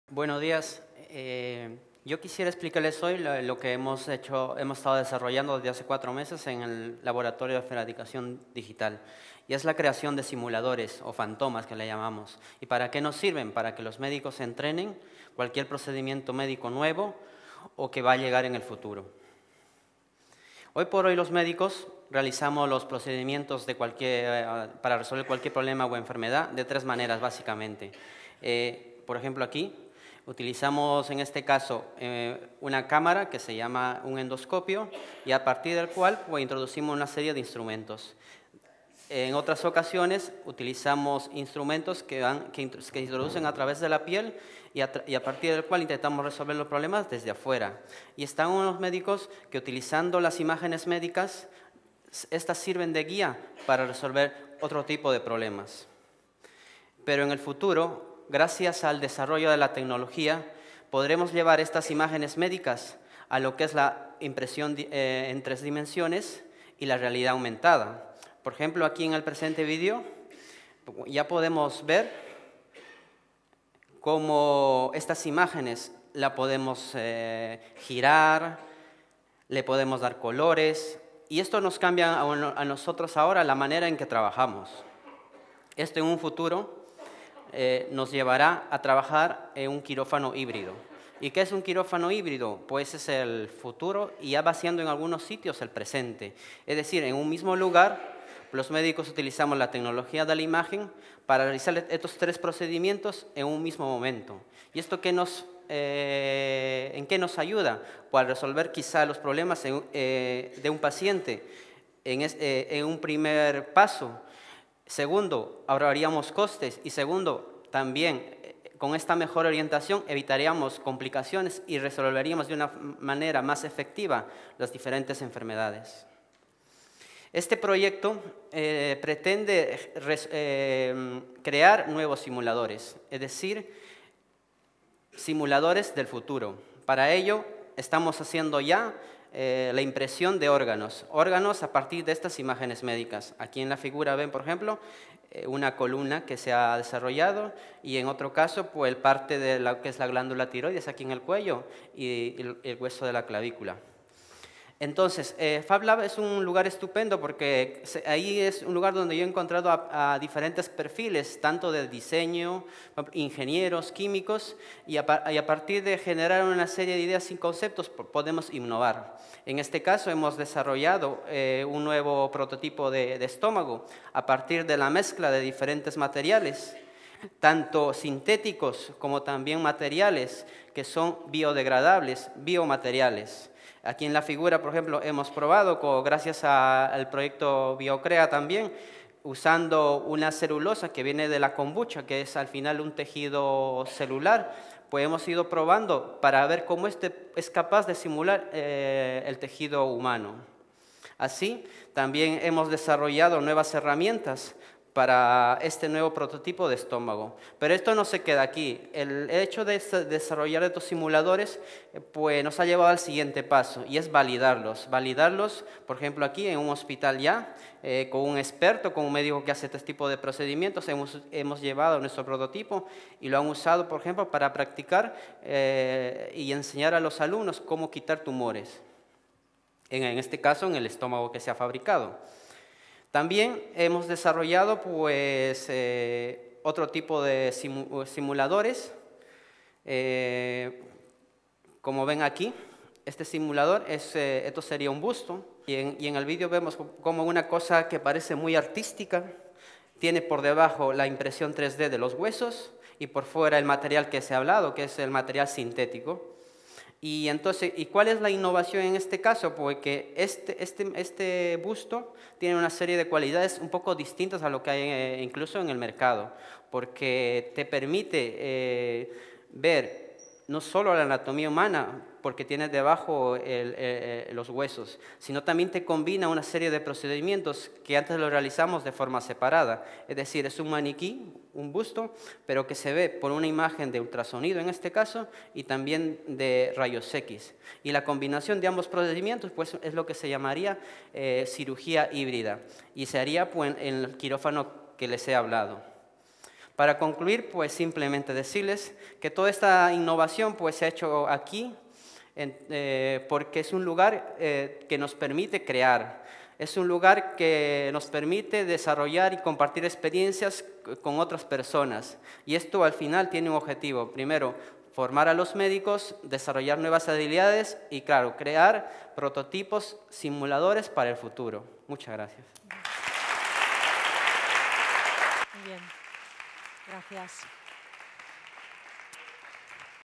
Desarrollo de modelos anatómicos (fantomas) para entrenamiento médico con impresión 3D
Este proyecto persigue diseñar y crear un modelo de simulación de estómago, duodeno y vías biliares a partir de una imagen médica, usando la impresión 3D y que sirva para el entrenamiento endoscópico y la adquisición de habilidades intervencionistas para médicos.
Lo cuenta Ernesto Barzola durante las charlas de Un año en un día, conducidas por Antonella Broglia, en las que se dan a conocer proyectos destacados del año en Medialab Prado.